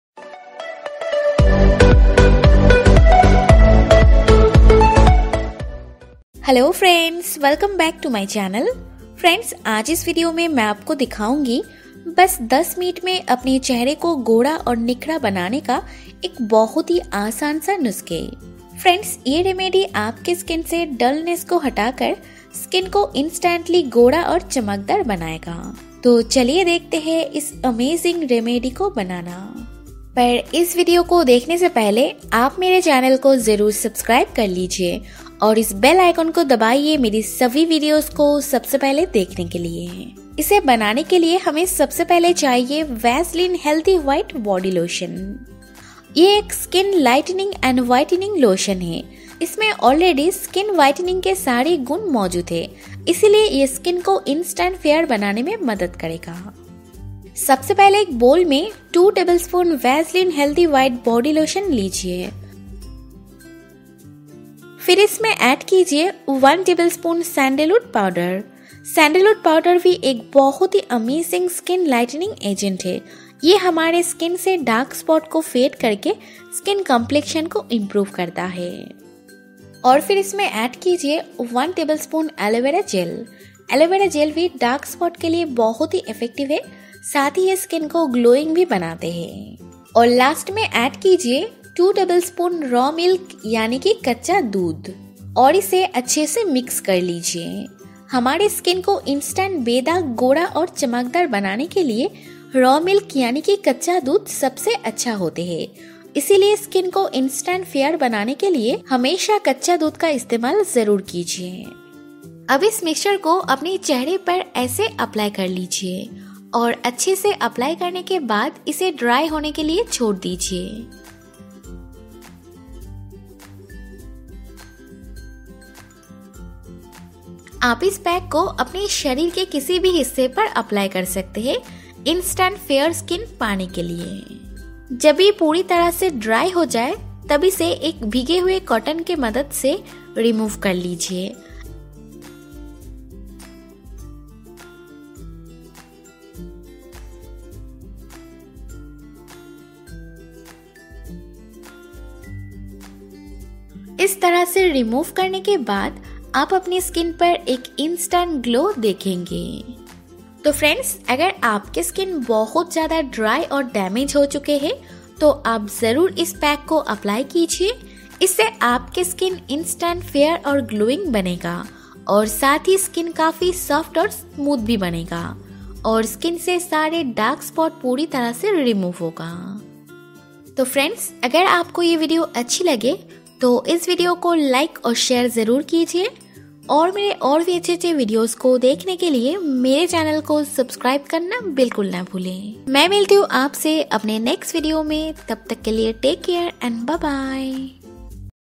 How to Use Vaseline Body Lotion For Skin Whitening | Vaseline For Skin Whitening Formula
To day in this video i will share with you an Amazing Magical Skin Whitening Formula with Vaseline Healthy White Body Lotion.

In Just 10 Minutes you can definitely see the amazing results.

Grow your Hair 2-3 inches in a week | Surprising Hair Growth Treatment"